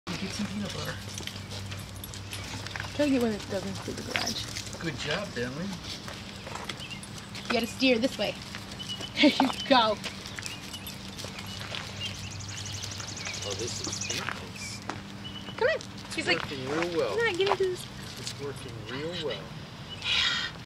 Bentley on wheels
Bentley has DM and is becoming paralyzed so we built him a wheel chair. He he is testing it out for the first time. We had to make some modifications after this and he is now much more comfortable so he walks much better in it now.